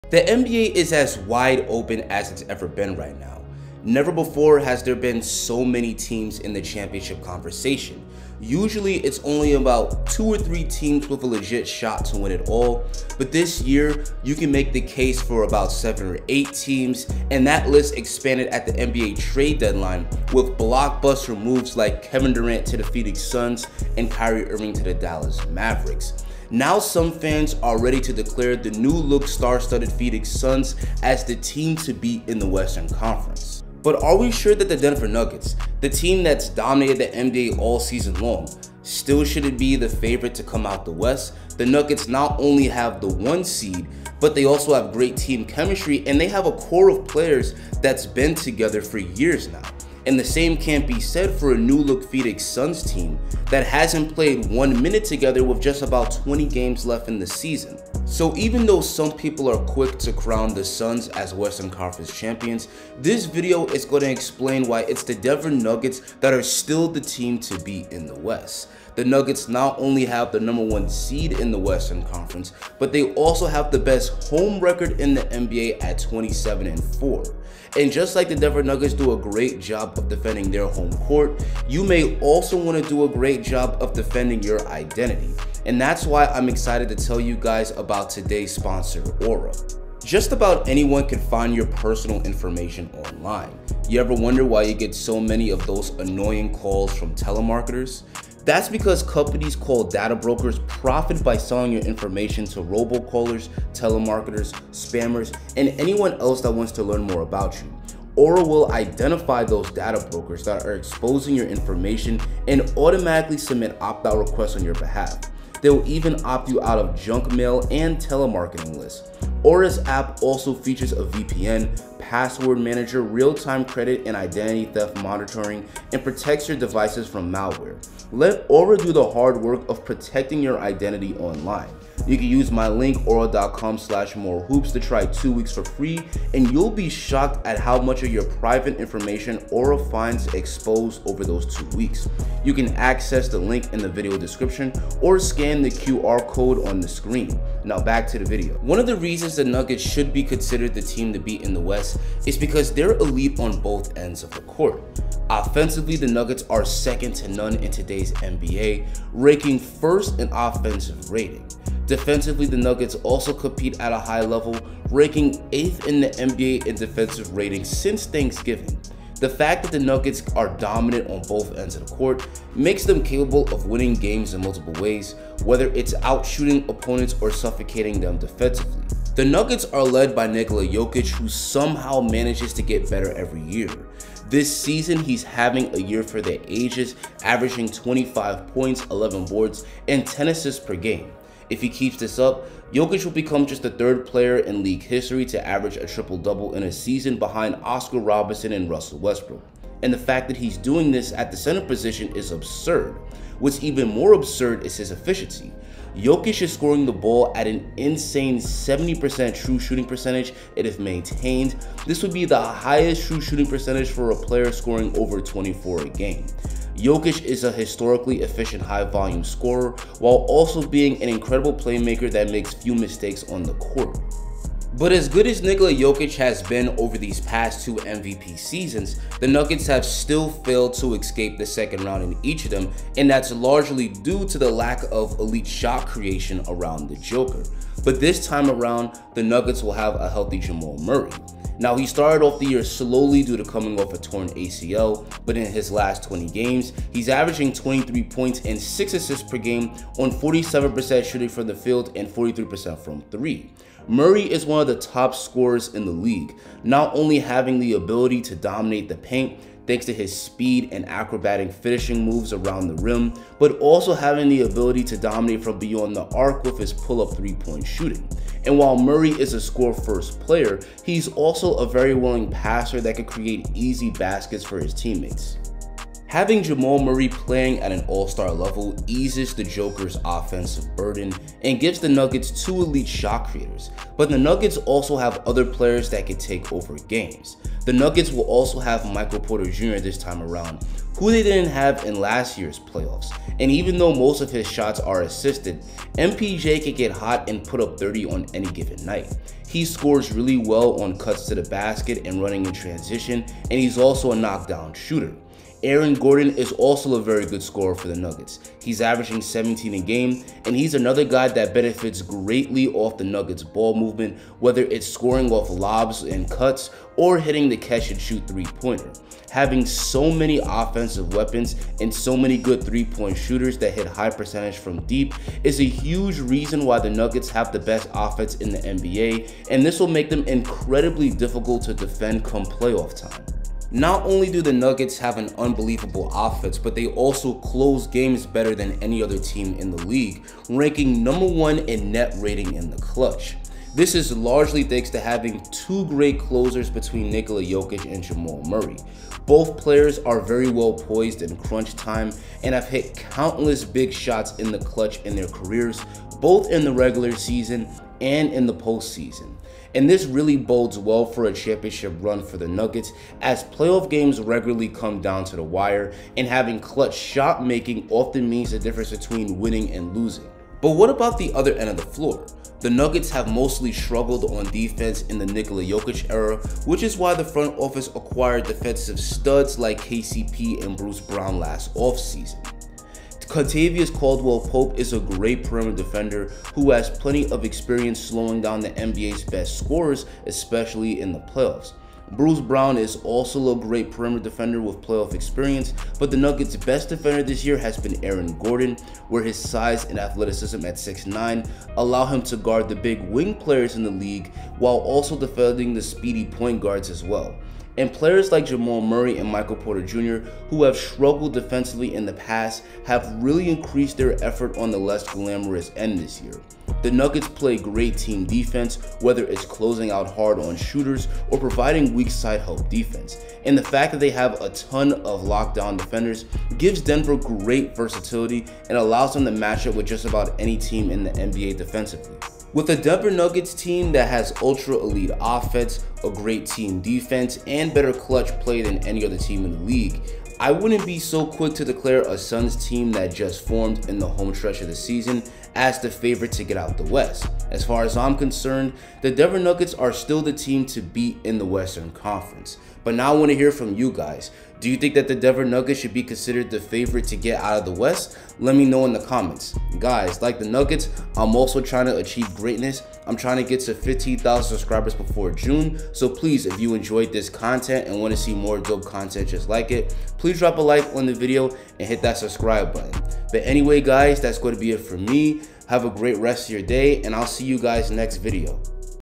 This Is Why The Denver Nuggets Are Still the Team to Beat in the West
The NBA is as wide open as it’s ever been right now. Never before have so many teams been in the championship conversation. Usually it's only 2 or 3 teams with a serious chance to win it all but throughout this year you can make this case for about 7 or 8 teams. That list expanded at the trade deadline with blockbuster trades like KD to the Suns & Kyrie to the Mavs. Already some fans are declaring the star studded Suns as the team to beat in the West. This video will explain why the Denver Nuggets are actually the team to beat in the West.